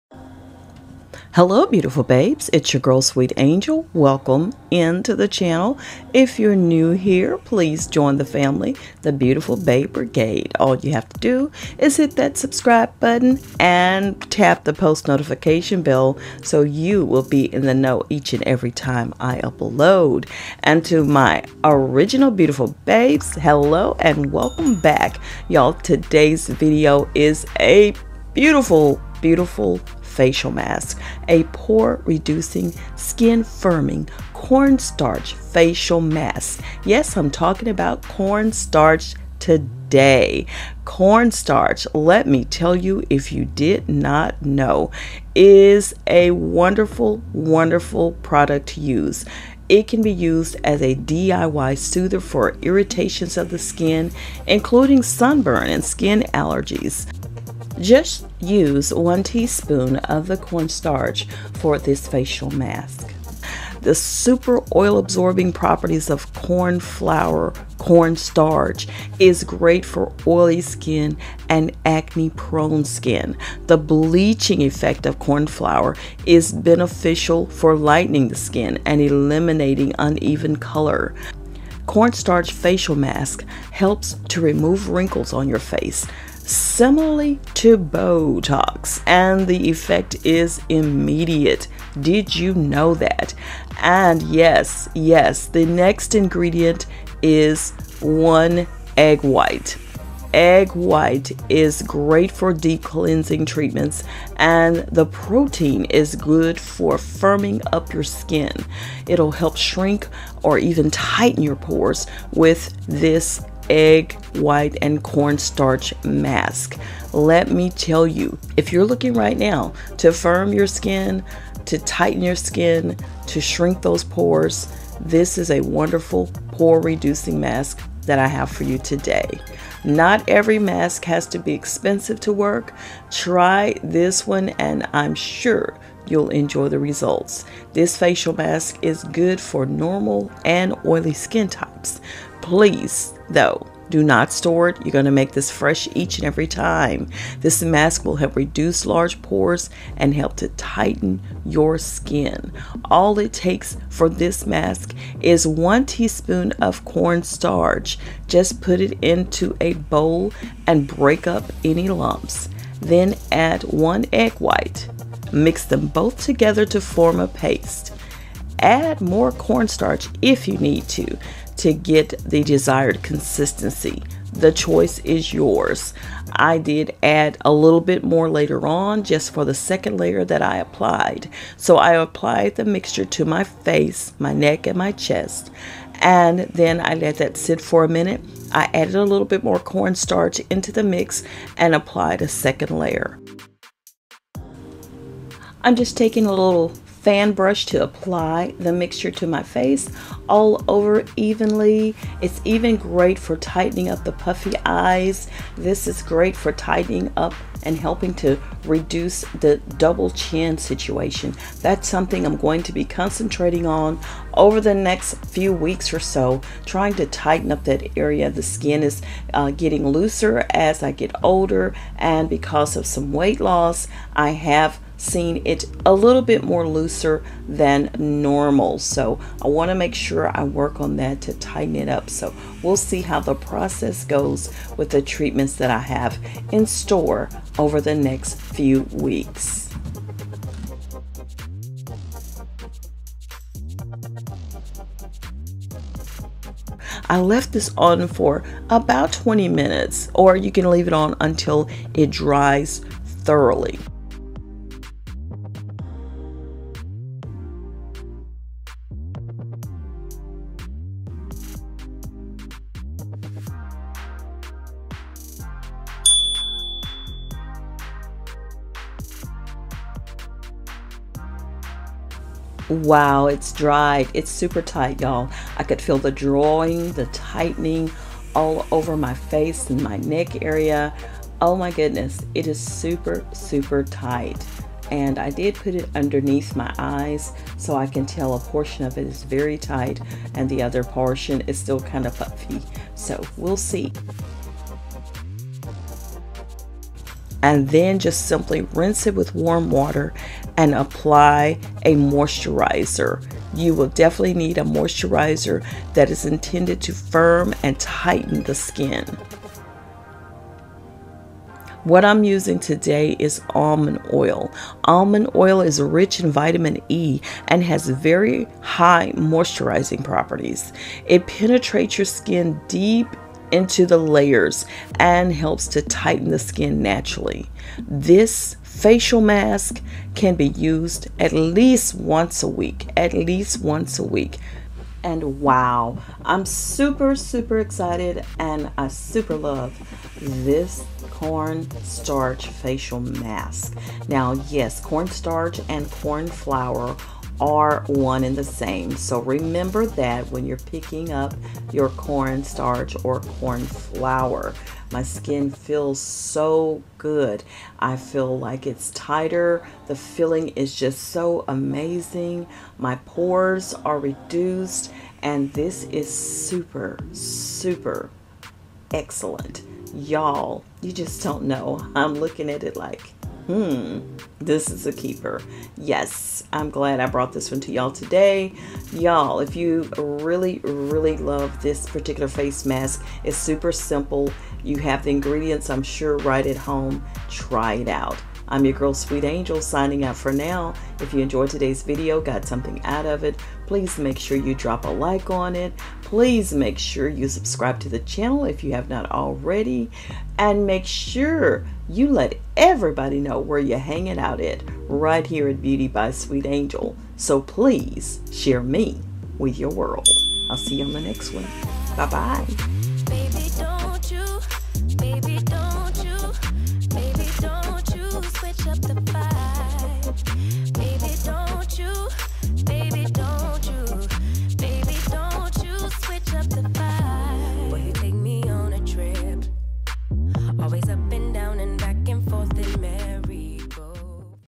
Cornstarch Facial Mask for Tightening Skin and Minimizing Pores
I have a wonderful pore reducing mask for you today.  Not every mask has to be expensive to work. Try the following and I'm sure you'll enjoy the results. This facial mask is good for normal and oily skin types. DO NOT STORE!
This mask will help reduce large pores and help tighten your skin.

Cornstarch in a face mask:  Cornstarch can be used as a DIY soother for irritations of the skin, including sunburn and skin allergies. The super oil-absorbing properties of corn flour/cornstarch is great for oily skin and acne-prone skin.  The bleaching effect of cornflour is beneficial for lightening skin and eliminating uneven color. Cornstarch facial mask helps to remove wrinkles on your face, similarly to Botox, and the effect is immediate.

Cornstarch contains Vitamin A which is responsible for color correcting dark spots on your face; it also contains iron and calcium which are responsible for skin cells renewal.

Egg white in face mask:  Eggs are great for deep cleansing treatments and the protein is good for firming up your skin. Shrink or tighten pores with an egg white face mask.

Corn Starch (2 lb.) by Pure Organic Ingredients, Cornstarch can be used as a DIY soother for irritations of the skin, including sunburn and skin allergies.

Pour 1 T. of Cornstarch (Argo Corn Starch 16 oz. Box into a bowl and break up any lumps.  Add 1 Egg White.  Mix both together to form a paste.  Add more cornstarch if needed to get the desired consistency.  Apply mixture to your face, neck, chest.  Leave on for 10-20 or until dries thorough.  Rinse with warm water and apply a moisturizer intended to firm and tighten skin. Use this at least once a week. I am using almond oil Almond oil is rich in vitamin E and has very high moisturizing property. It penetrates deep in the skin and tightens the skin naturally.